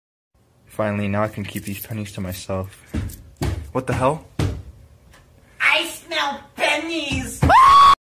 I SMELL PENNIES | Lust & Fell | Gacha Club Undertale AU | Remake!!
I had to remake this 😂
My brother was looking for my video and found it then I realized that I made it 10 months ago with my own audio and old Gachas
So I found the audio and made a video with it
Apps Used: Gacha Club, CapCut